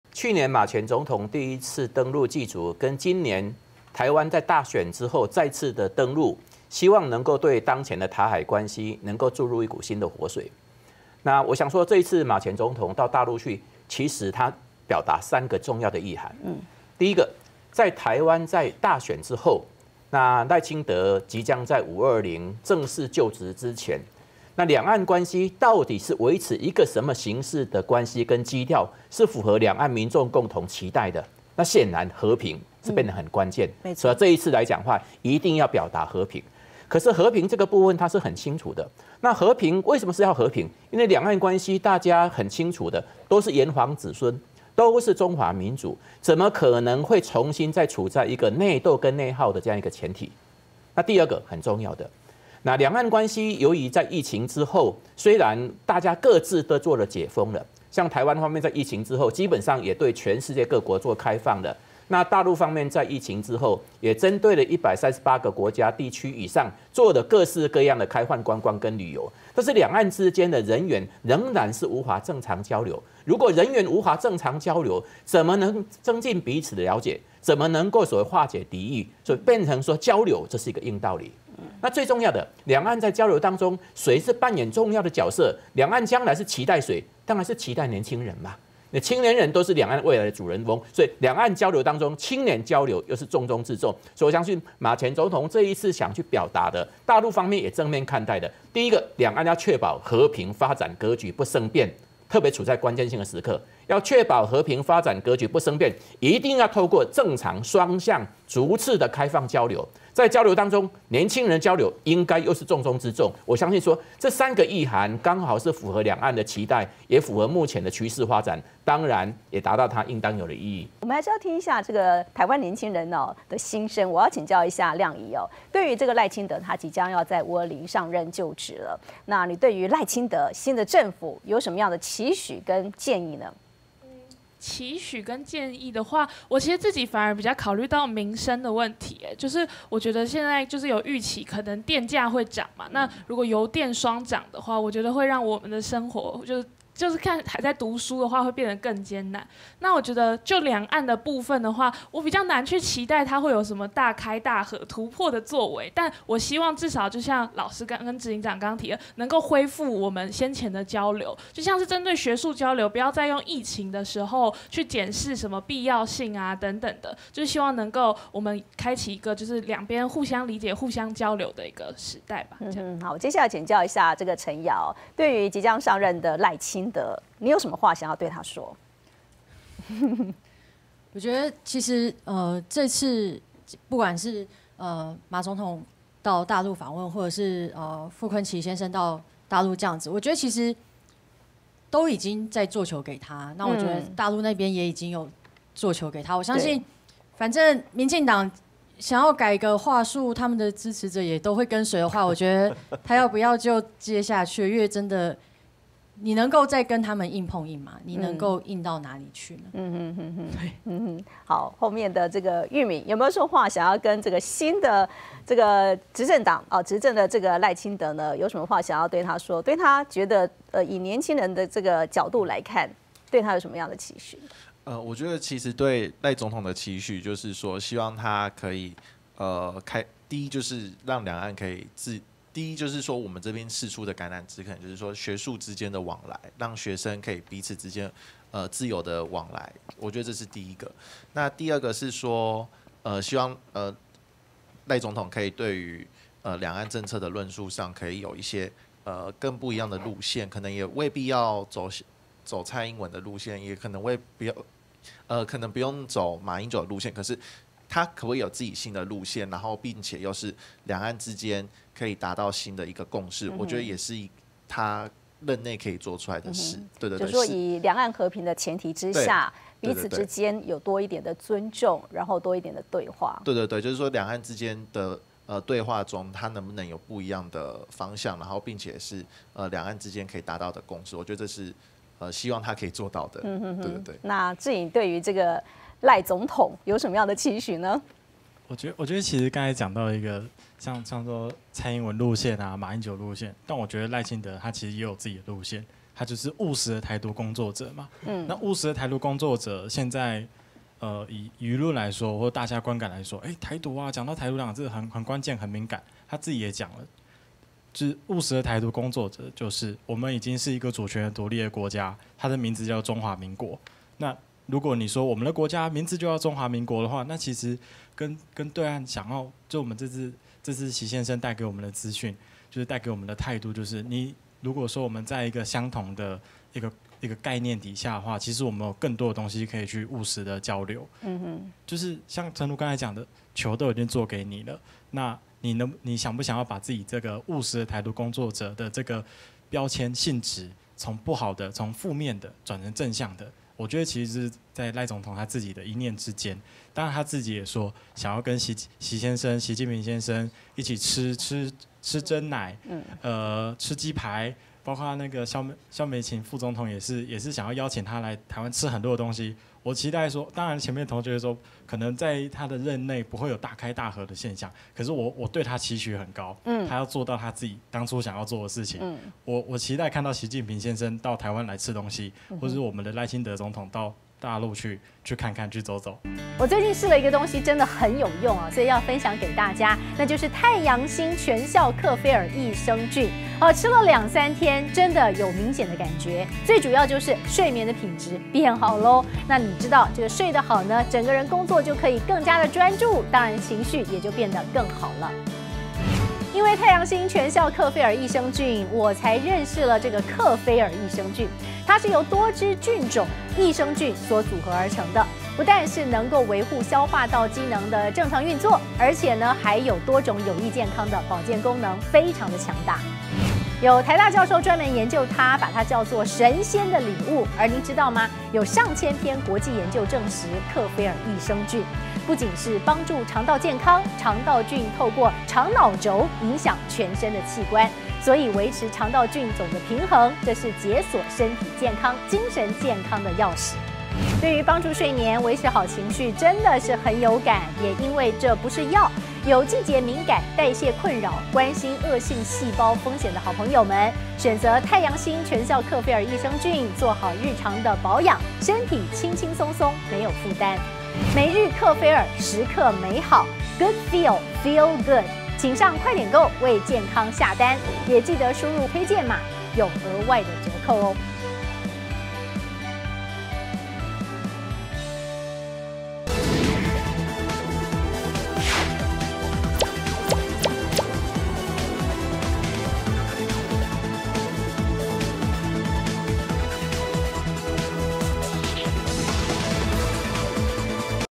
訪陸學子"喊話賴總統!"做到他說要做的事!兩岸和平躍進 "張五岳:"青年人是重中之重| cti talk網路論壇@中天電視CtiTv@ctitalk網路論壇
主講人：
馬英九基金會執行長 蕭旭岑
淡江兩岸中心主任 張五岳